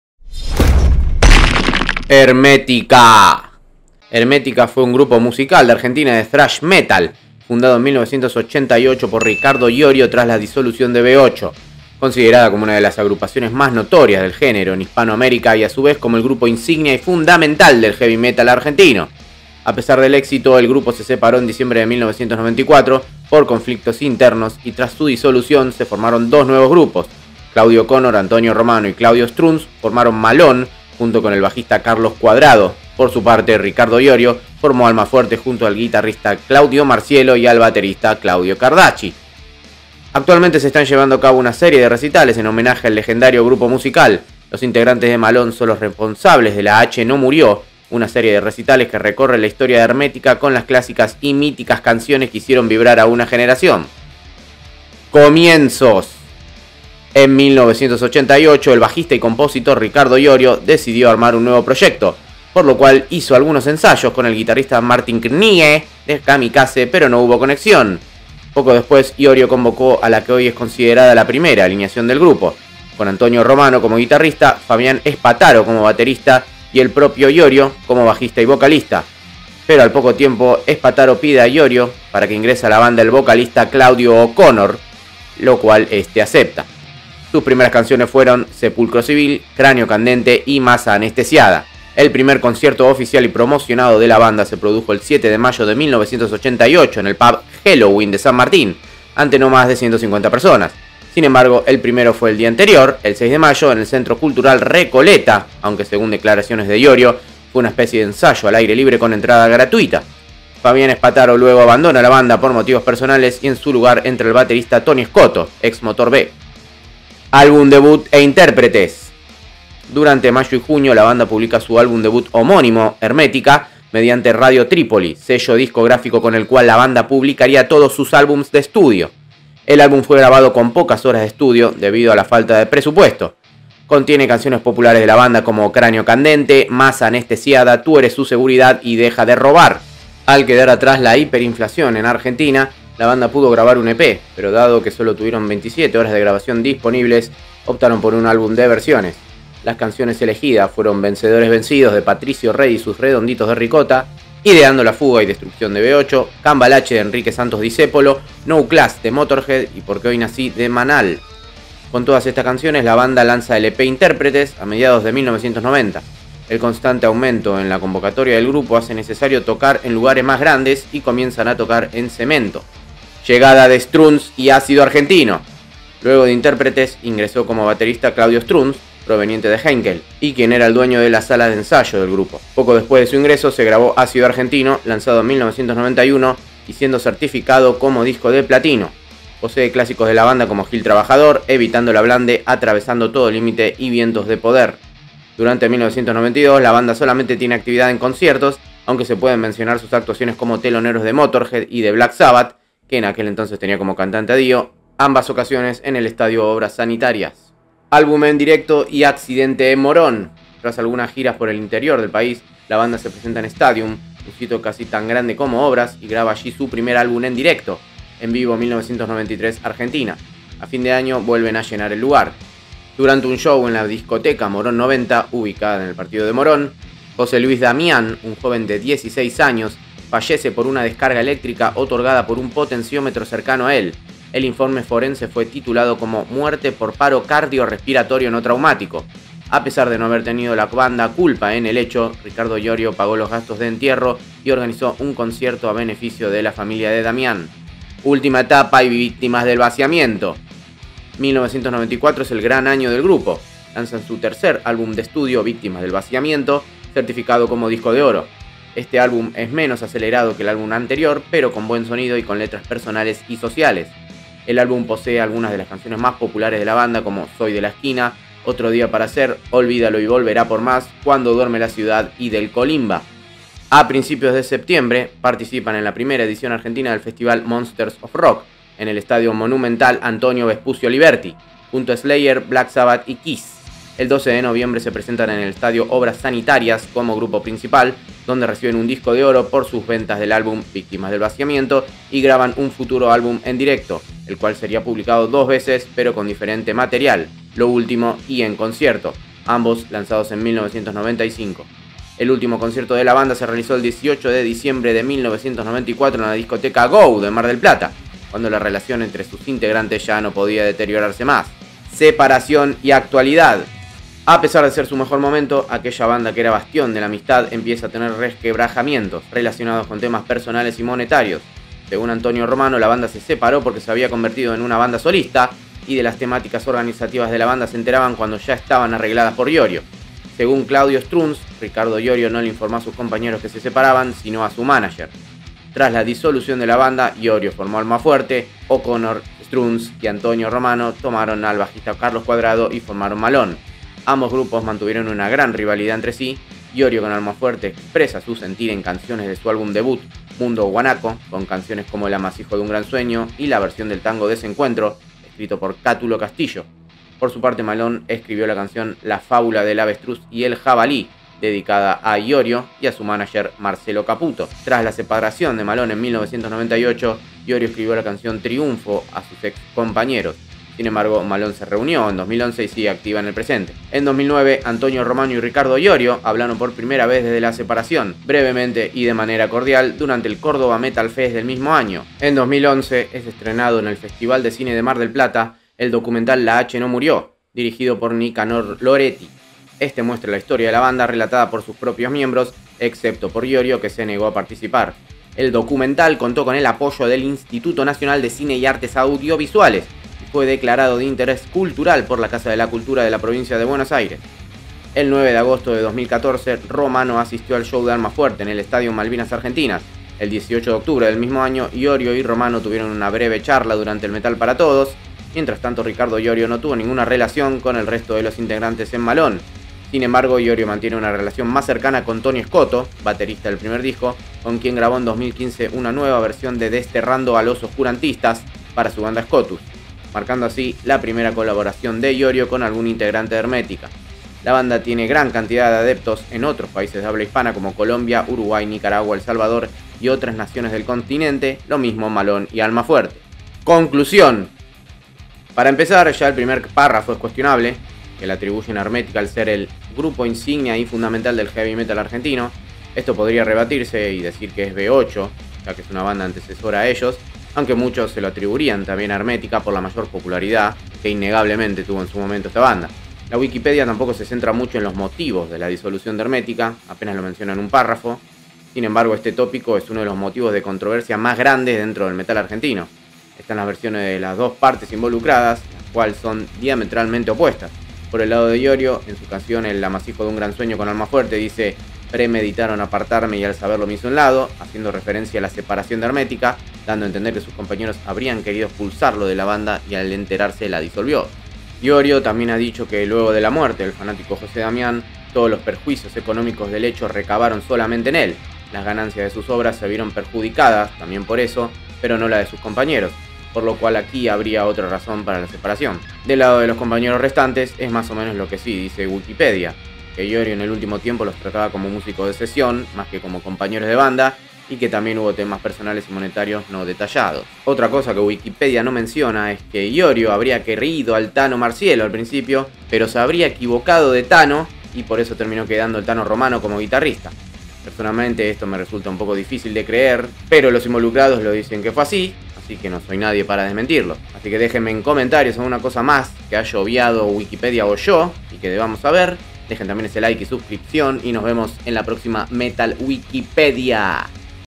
Conociendo a Hermetica: Historia de una banda legendaria
Nueva entrega de METAL WIKIPEDIA dedicada a una banda legendaria del metal sudamericano: HERMETICA.

Repasamos lo que la Wikipedia cuenta sobre Hermetica, y tambien si cuentan todo, o solo parte de la historia.

Hermetica fue una banda de thrash metal argentina, que aunque duró pocos años, caló profundo en los metaleros de sudamerica.
Con solo 3 discos quedo en la HISTORIA como una de las leyendas del metal latino.
Descubri su historia, desde su fundacion hasta separacion, y la actualidad de sus integrantes

IMPORTANTE: Todo el contenido narrado fue extraido de Wikipedia, ese es el objetivo de esta sección, lo expuesto no supone la verdad ni la opinion de los integrantes del canal

CREDITOS:
VIDEOS UTILIZADOS: